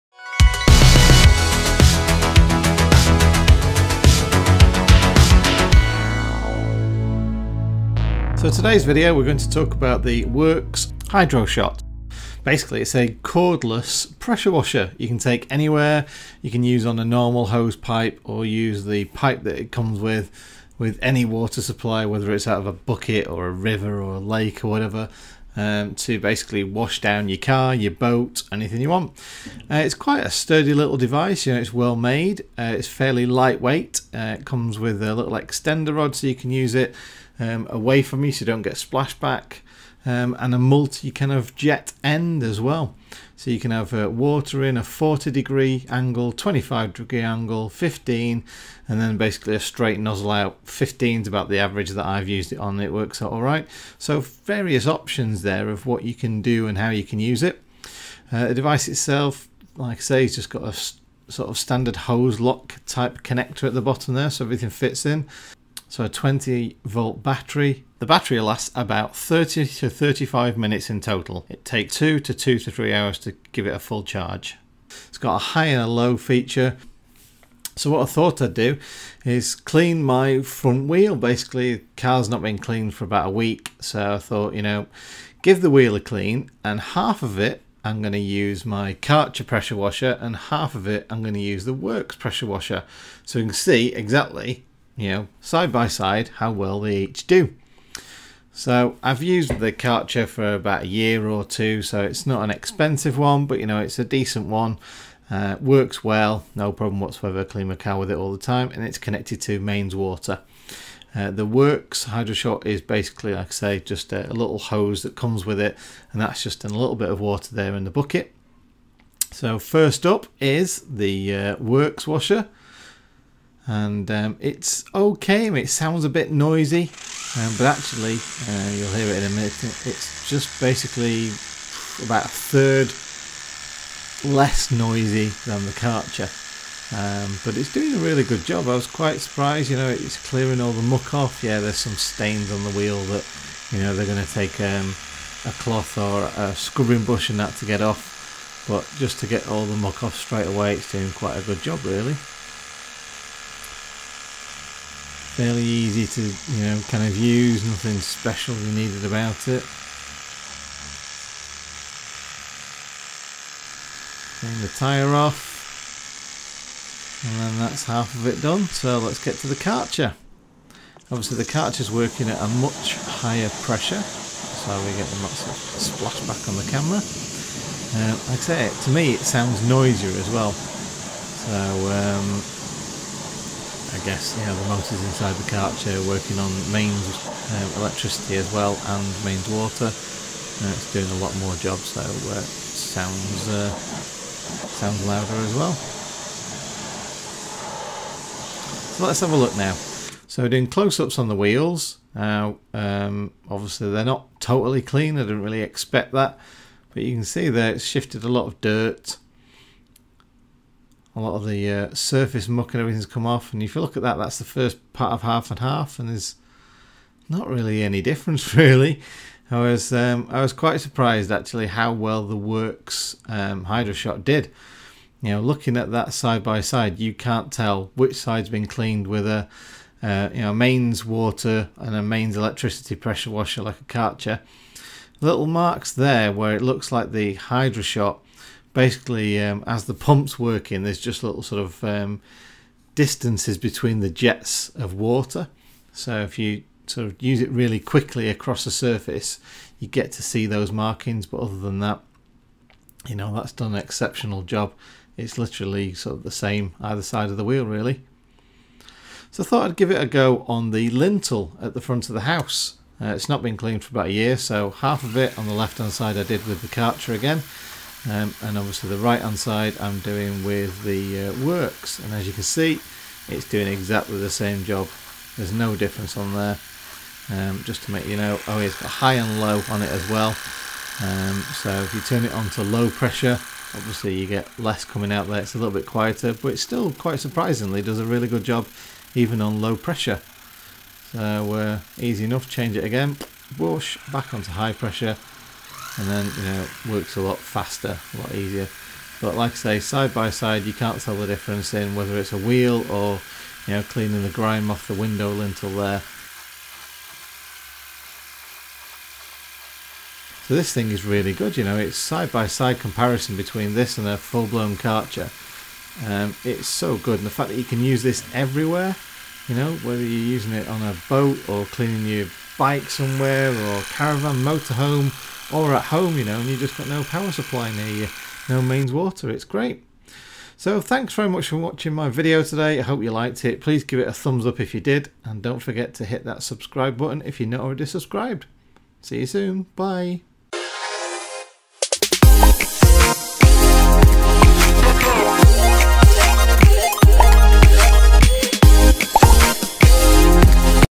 Worxs HydroShot Cordless Pressure Washer vs Karcher - Real Review
The Worx Hydro Shot is cordless pressure washer, take it anywhere, use any water supply to clean your Car, Bike, Boat, Caravan, Motorhome or whatever you need cleaning where you don't have easy access to electricity or a hose pipe.

To see just how good this is I tested it on my dirty car wheel and dirty window lintel against a Karcher mains powered pressure washer.

All views are my own, are not to be taken personally and not to be trolled, cos that's just not cricket folks!